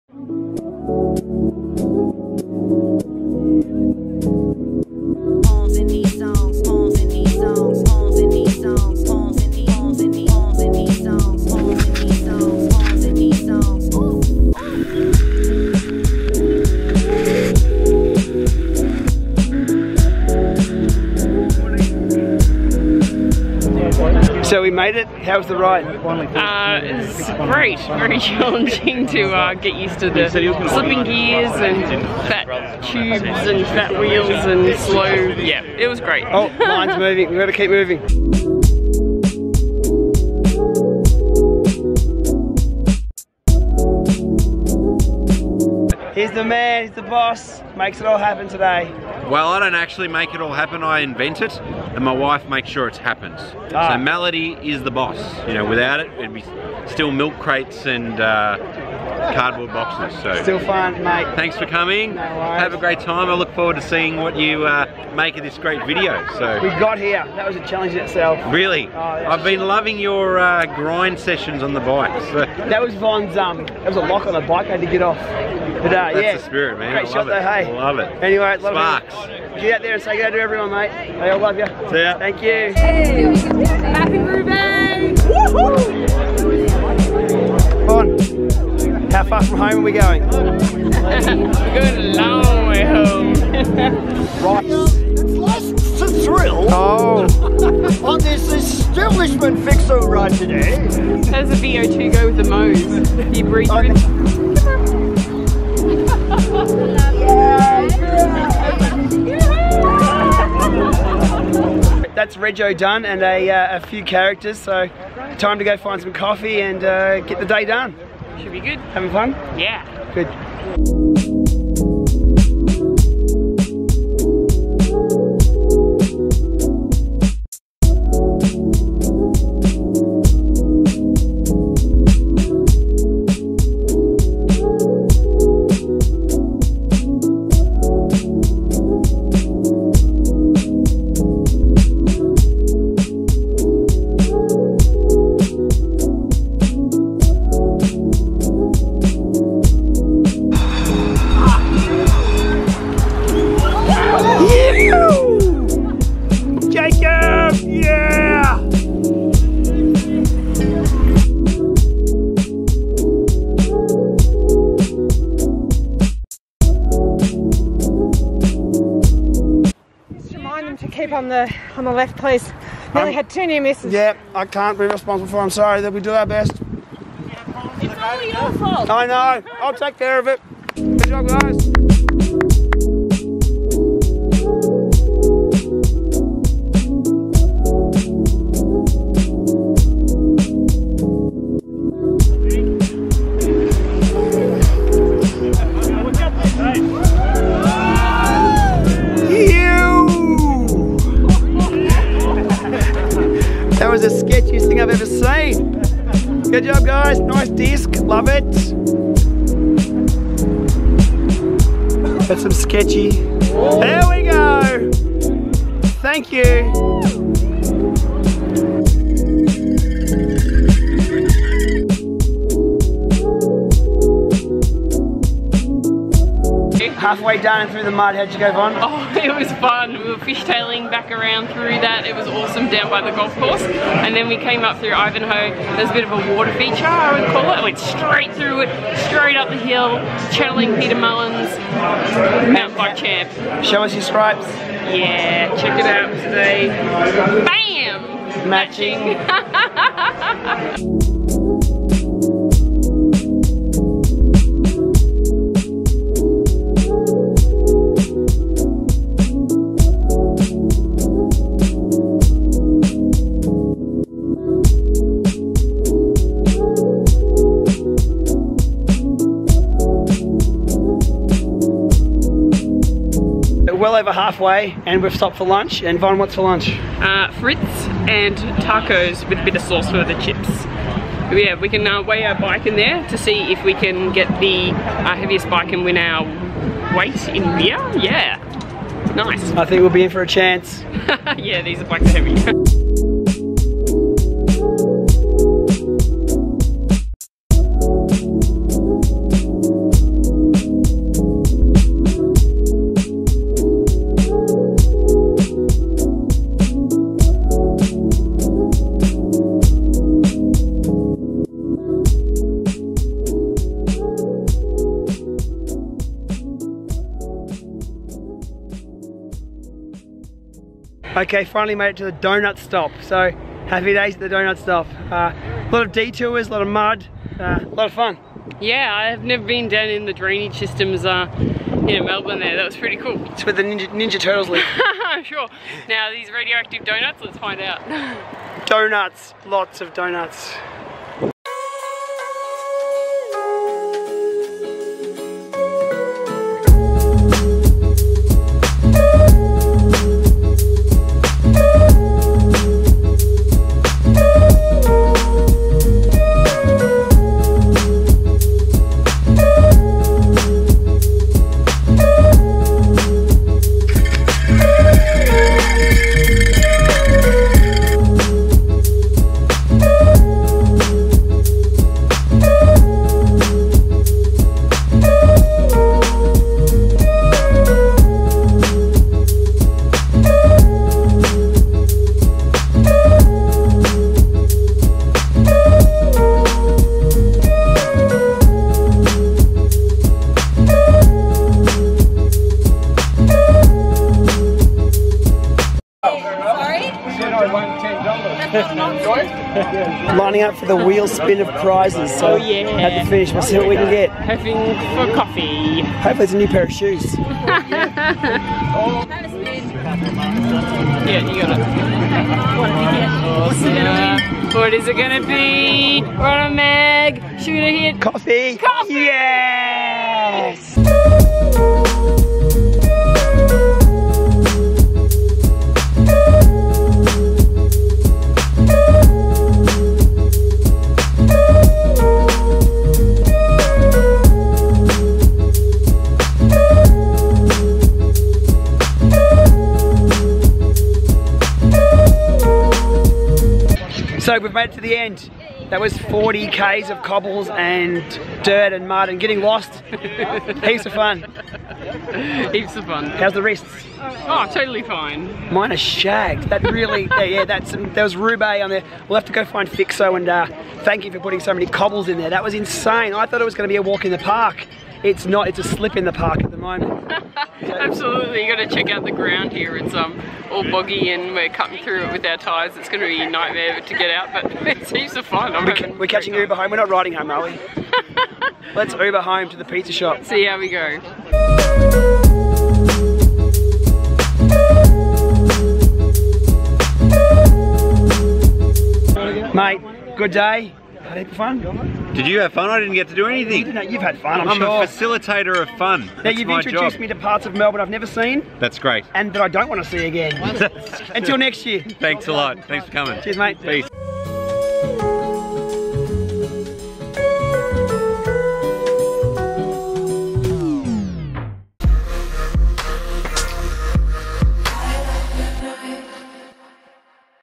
Melburn Roobaix Special 2016 - Daily Lama EP17
More fun than I could pack into a VLOG - The 2016 Melburn Roobaix presented by FYXO was a day to remember!